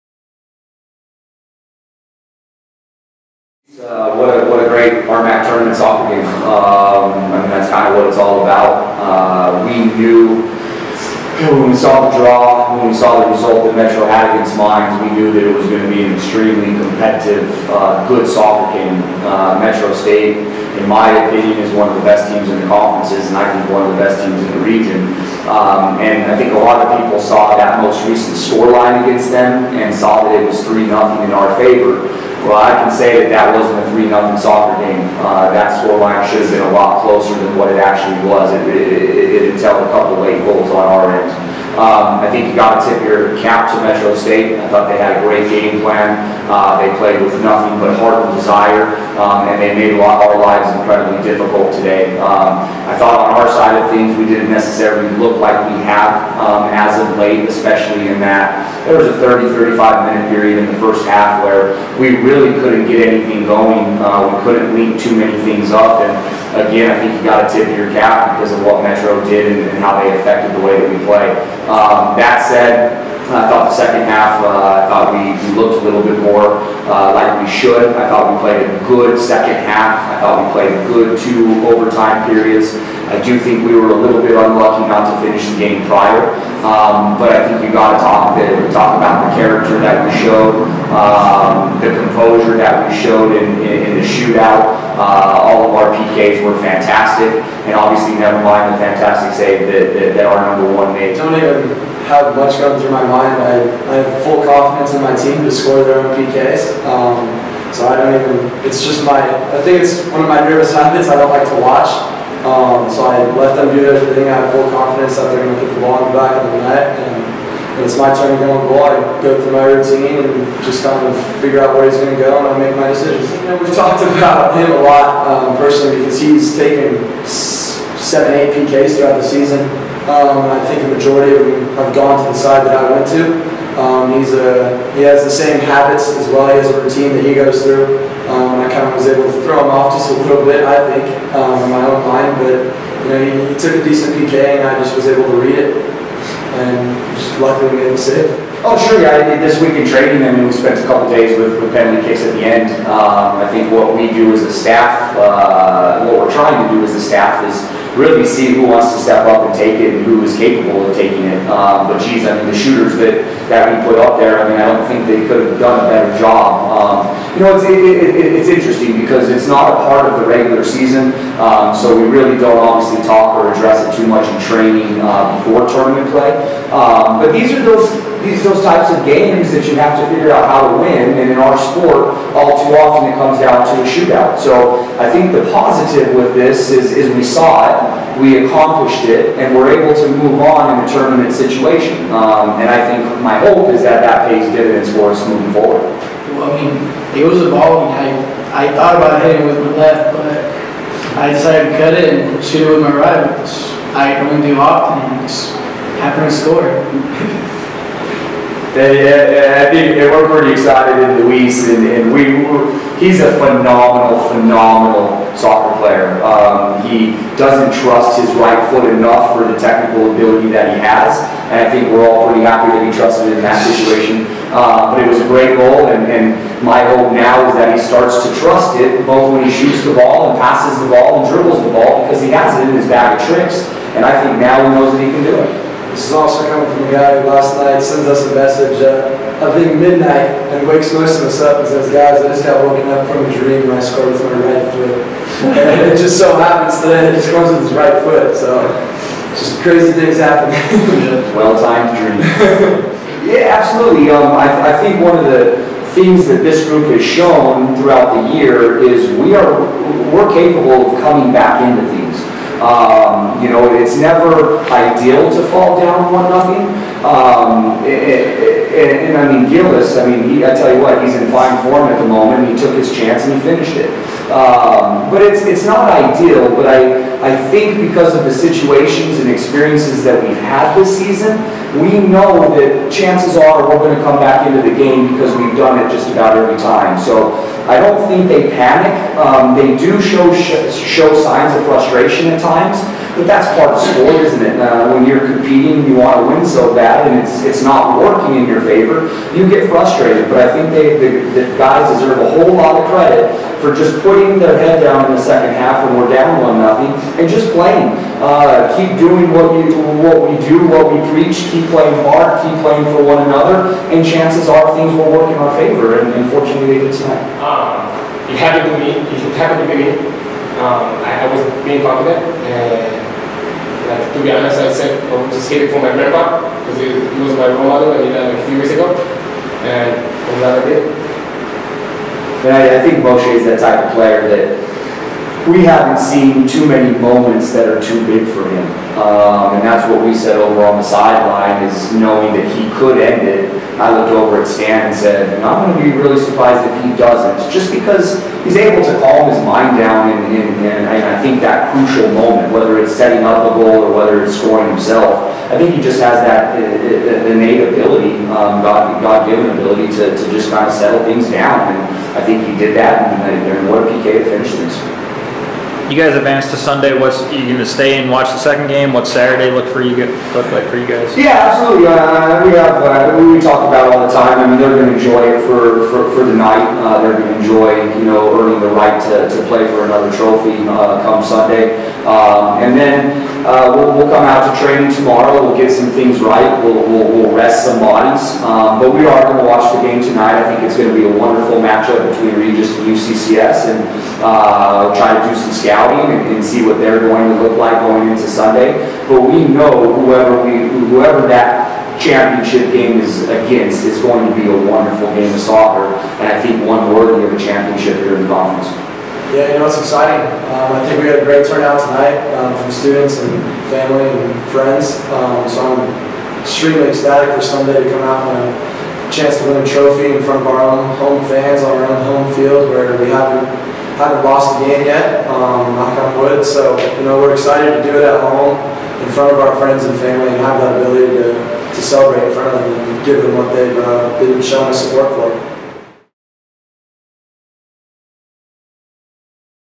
2016RMACmsoc Colorado Mesa Semifinal Postgame Interview
The Colorado Mesa men's soccer team defeated MSU Denver 4-1 in a shootout, after a 1-1 tie in regulation in the semifinals of the RMAC soccer tournament.